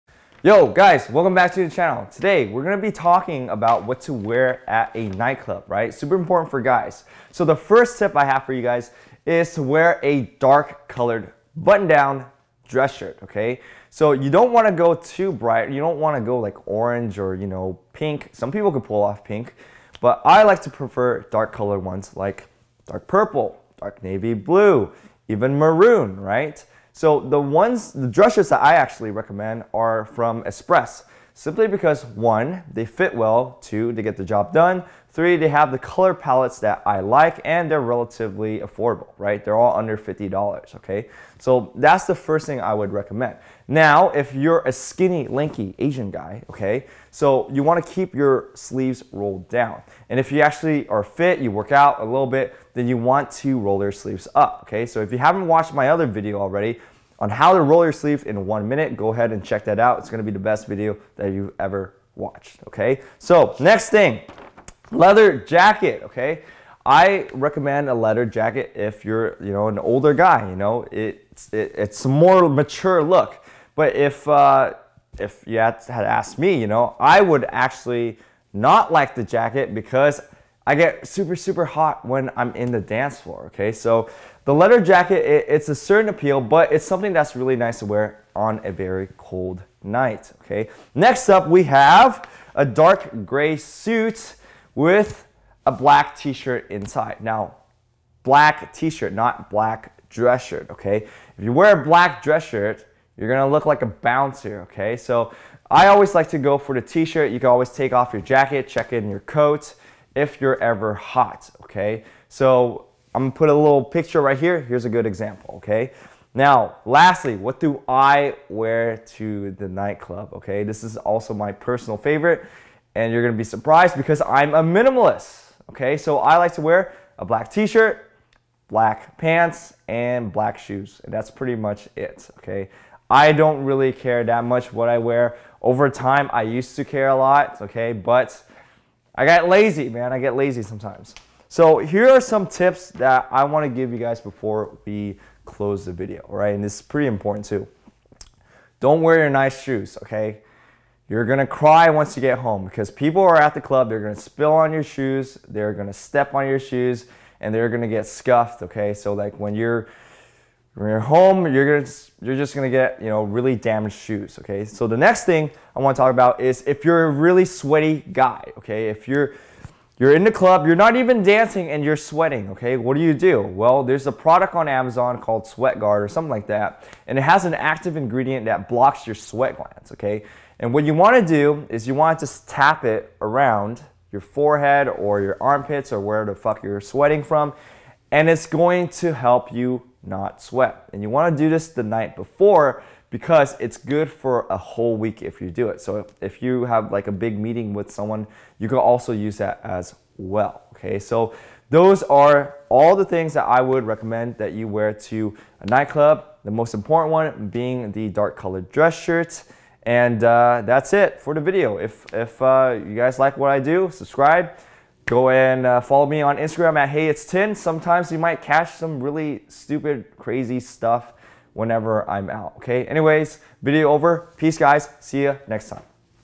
What to Wear to a Nightclub for Men
What you wear to a nightclub matters. This video will cover some quick tips and style recommendations for men.

1) Just wear a classic button down dress shirt that fits your body type
2) Go for darker color tones because it'll fit more in darker atmosphere of a club
3) Don't wear striped dress shirts and make sure they're solid color
4) Untuck your shirt for a more casual look
5) Roll up your sleeves for a sharper look

Straight to point as usual guys